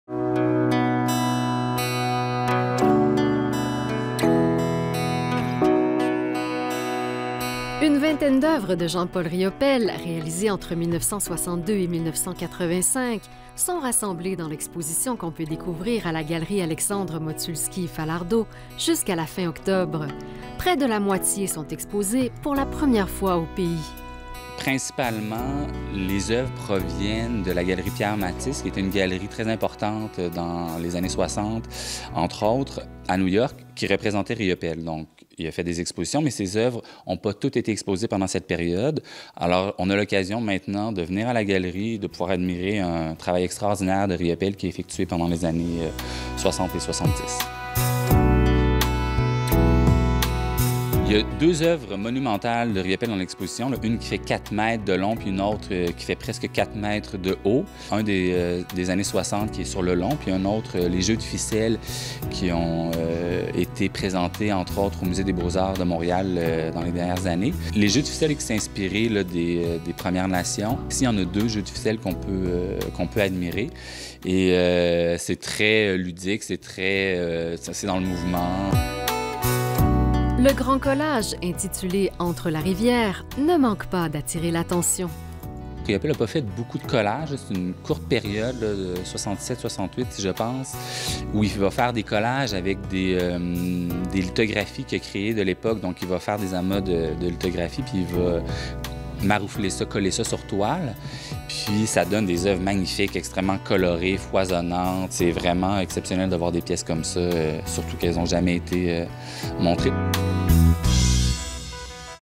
Jean Paul Riopelle : des œuvres exposées pour la première fois au Canada
La galerie Alexandre Motulsky Falardeau située sur la côte Dinan à Québec présente plus d’une vingtaine d'œuvres originales de Jean Paul Riopelle, dont plusieurs exécutées au fusain, à l’acrylique, à la gouache, à l’encre et au pastel.

Pour en savoir plus : rc.ca/SZCSkN

Suivez ICI Québec sur différentes plateformes